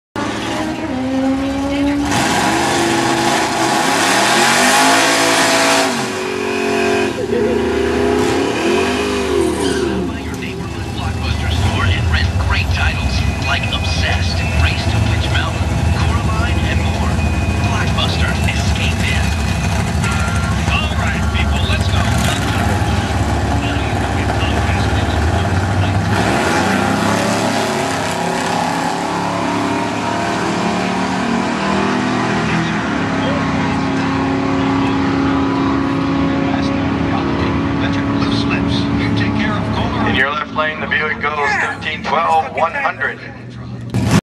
Milan Dragway NO ET 2009 Part 2
2009 Arm Drop Live @ Milan Dragway, MIchigan Pontiac Boyz (Shit Starters)